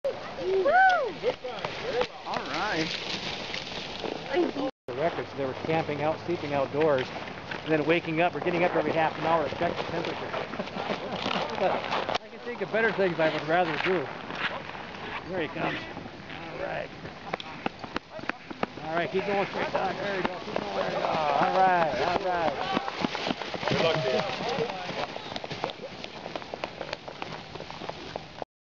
Beargrease Sled Dog Marathon - road crossing
See two teams cross the road in the Minnesota woods. Towards the end, you can hear the musher say "on by" to his team.